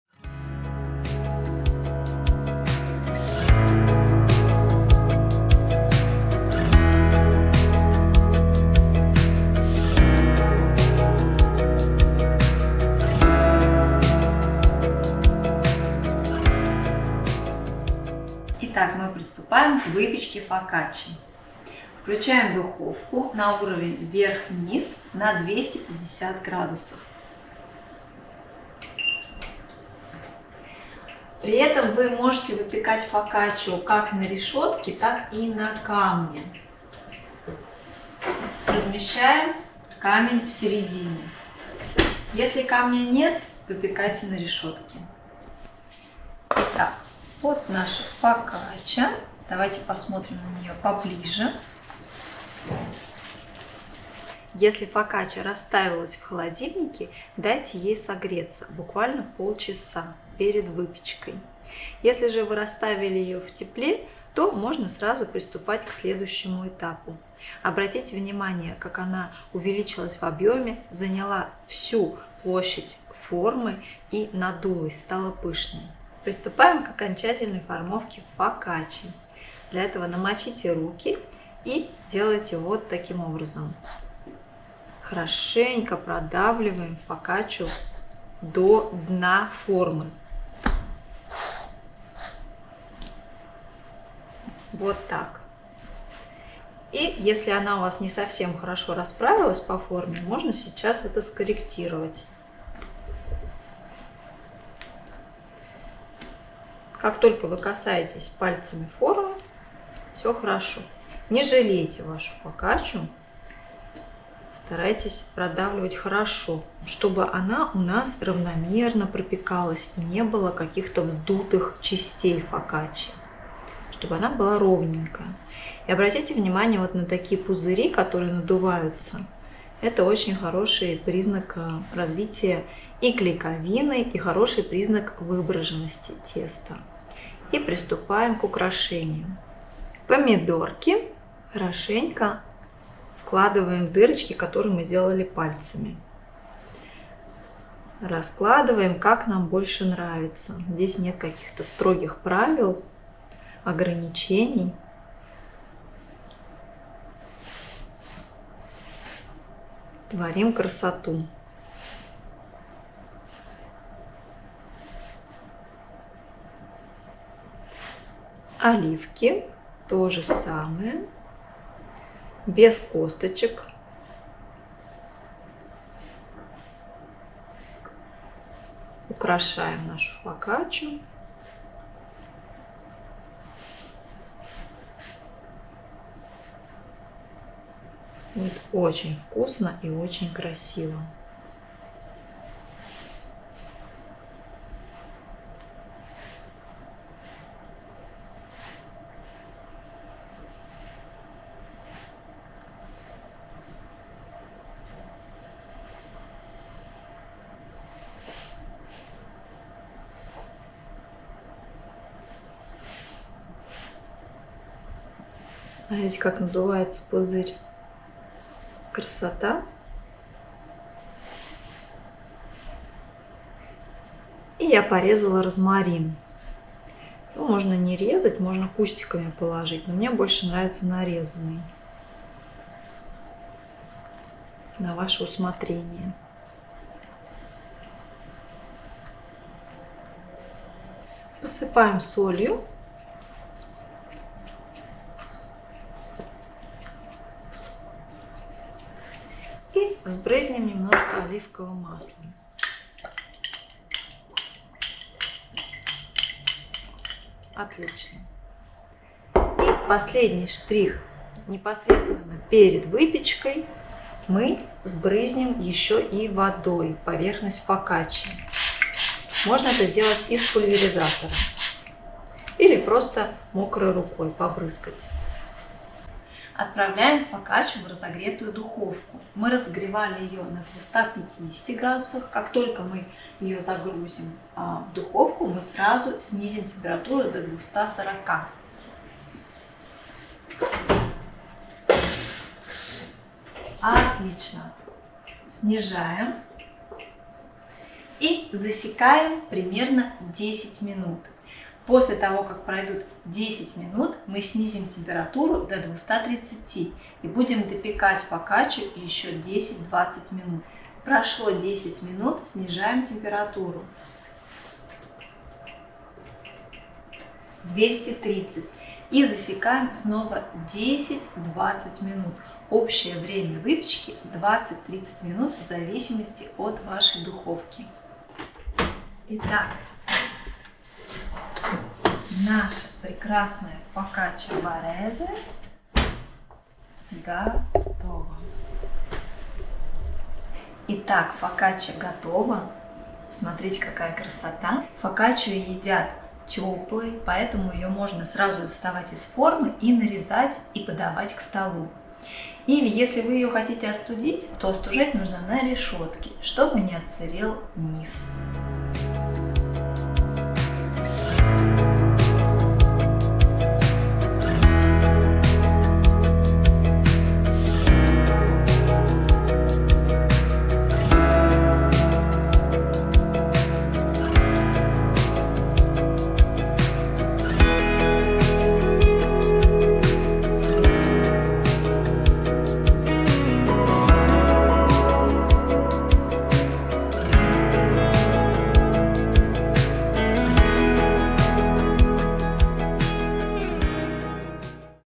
Вкуснейшая итальянская ФОКАЧЧА! Формовка и выпечка
Регистрируйтесь на бесплатный мастер-класс по итальянской выпечке на закваске с нуля

Формовка и выпечка фокаччи | Оh My Bread

Сегодня мы приступим к выпечке фокаччи. Её можно выпекать как на решётке, так и на камне.

Посмотрите на фокаччу поближе. Если она расстаивалась в холодильнике, дайте ей согреться буквально полчаса перед выпечкой. Если же вы расстаивали её в тепле, то можно сразу приступать к следующему этапу.

Обязательно посмотрите видео. Вы увидите весь процесс формовки, выпечки и украшения фокаччи. Не упустите ньюансы и сможете порадовать себя и своих близких вкусной итальянской лепёшкой.